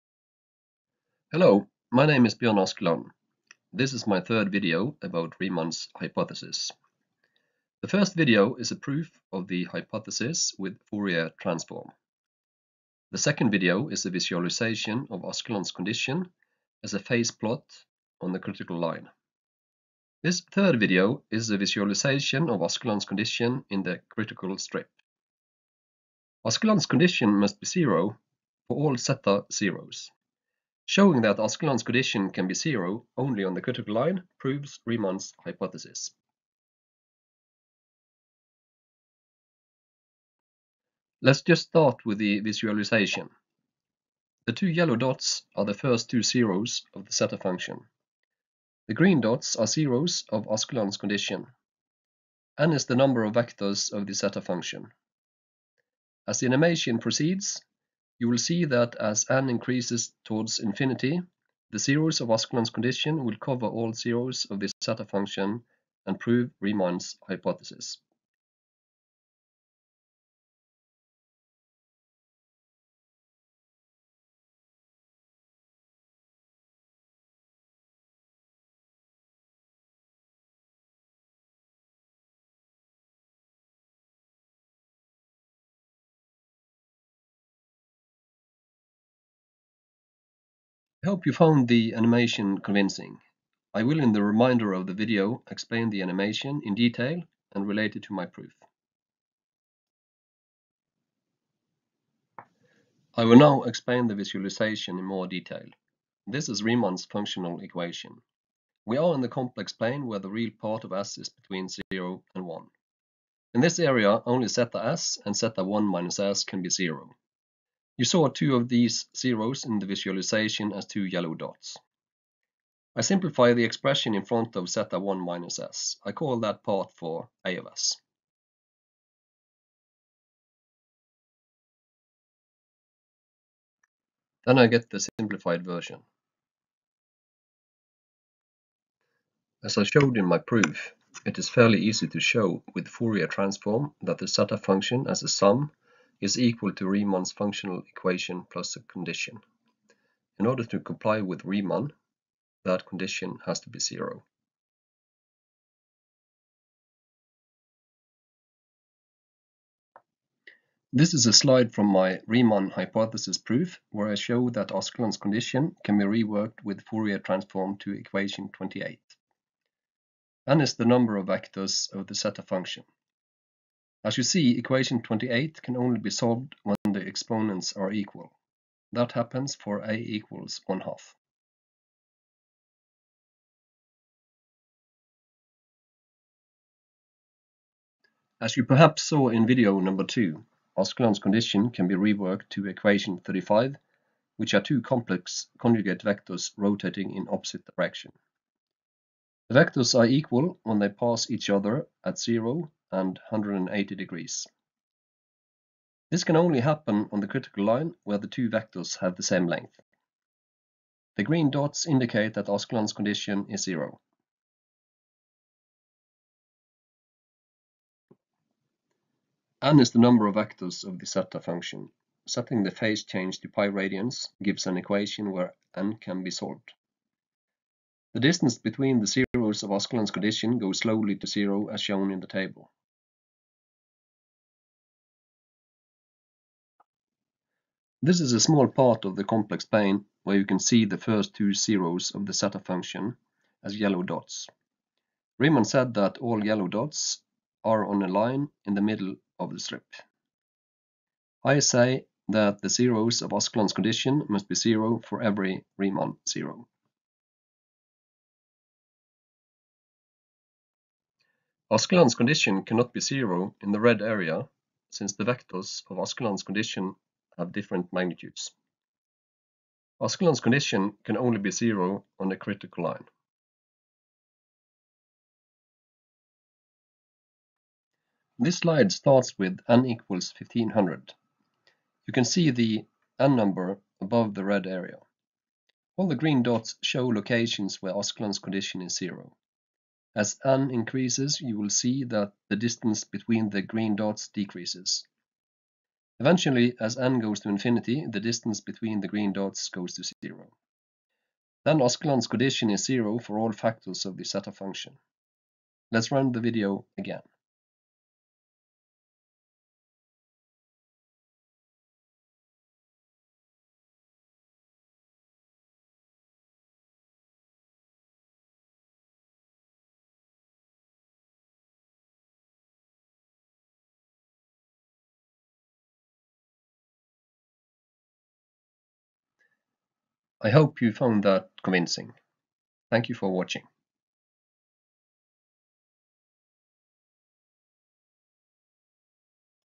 Riemann hypothesis, Askeland's Condition second visualization
Visualization of Askeland's condition which proves Riemann's hypothesis.